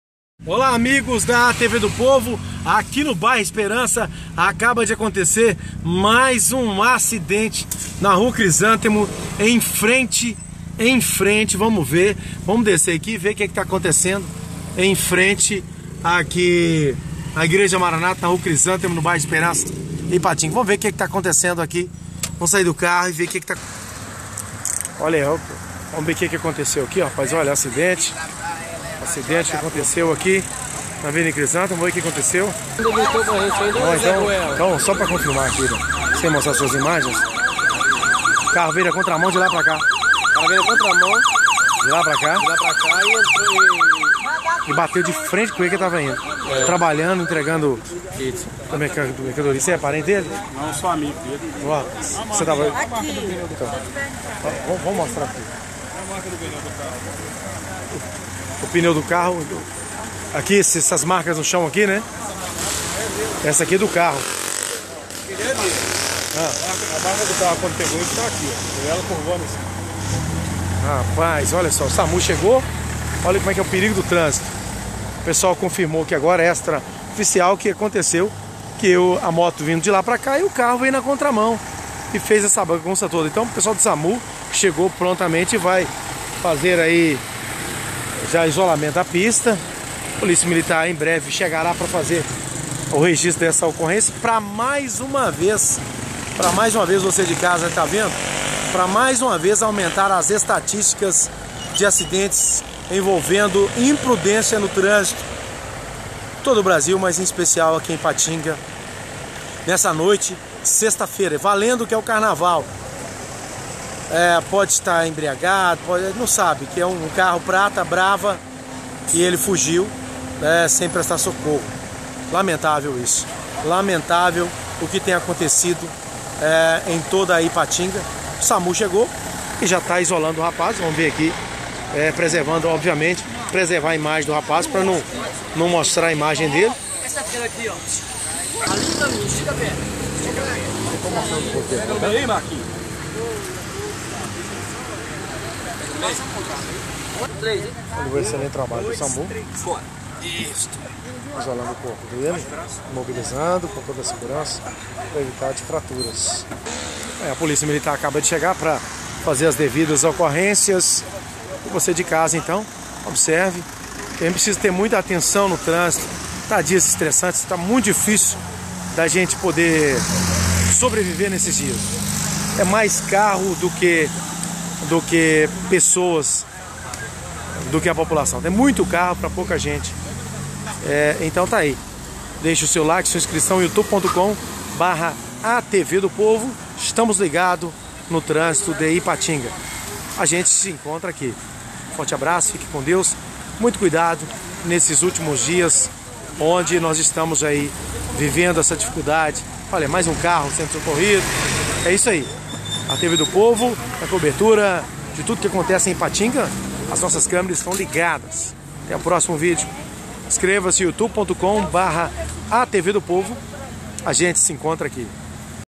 CARRO NA CONTRA MÃO ATROPELA MOTOQUEIRO NA RUA CRISÂNTEMO NO ESPERANÇA IPATINGA MG ATVDOPOVO
YOUTUBE.COM/ATVDOPOVO